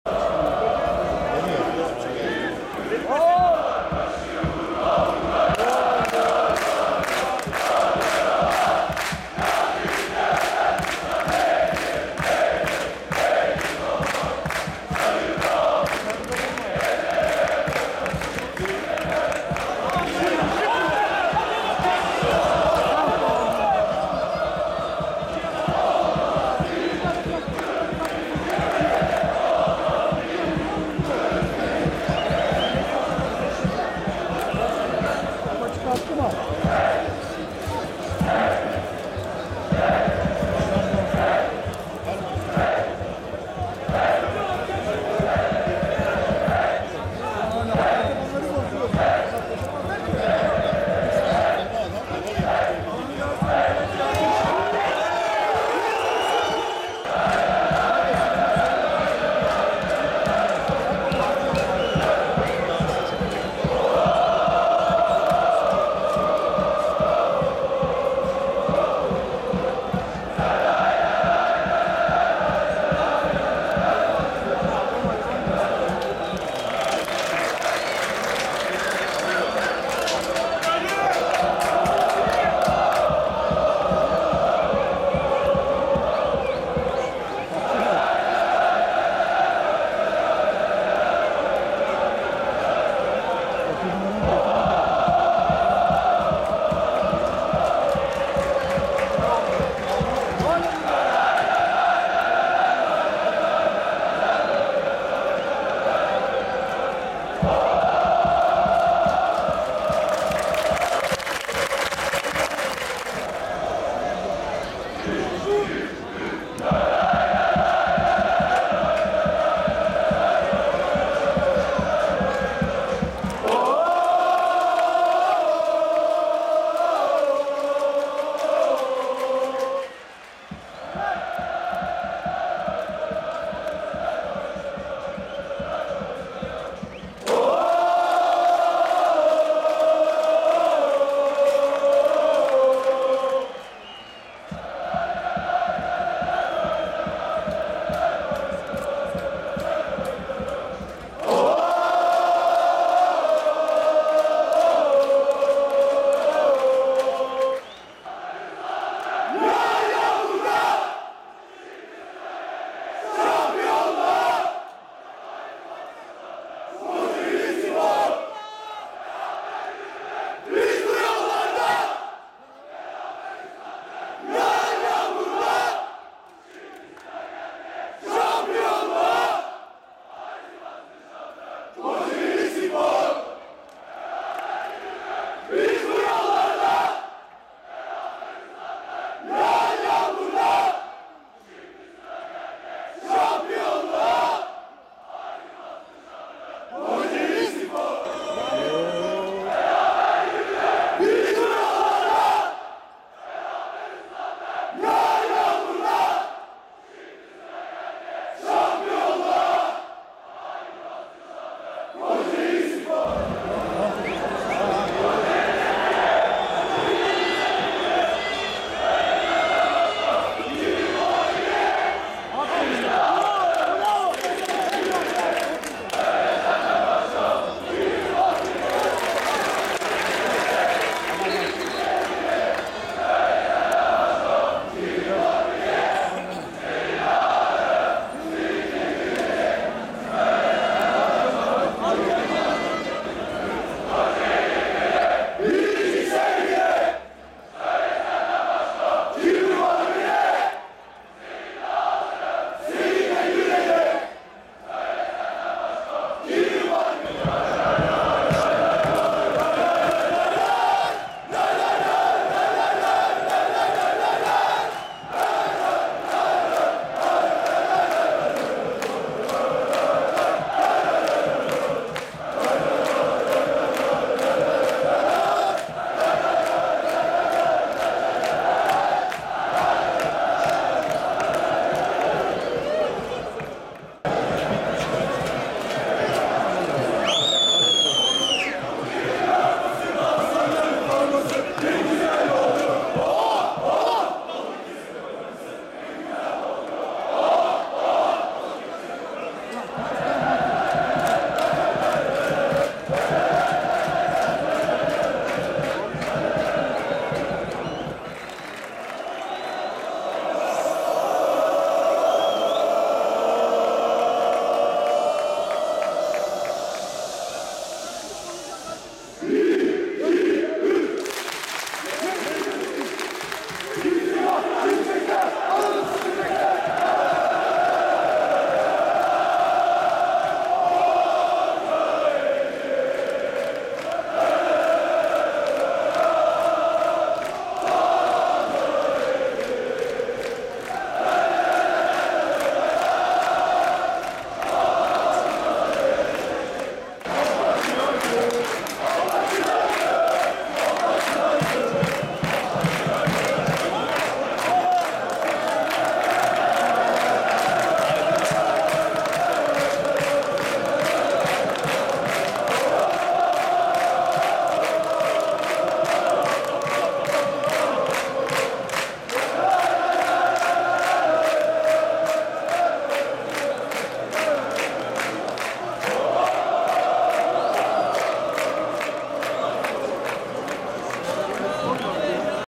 KOCAELİSPORUMUZ-B.KESİRSPOR MAÇTAN GÖRÜNTÜLER 2.BÖLÜM
KOCAELİSPORUMUZ:2-BALIKESİRSPOR:0 MAÇTAN TARFTARIMIZIN MÜTHİŞ GÖRÜNTÜLERİ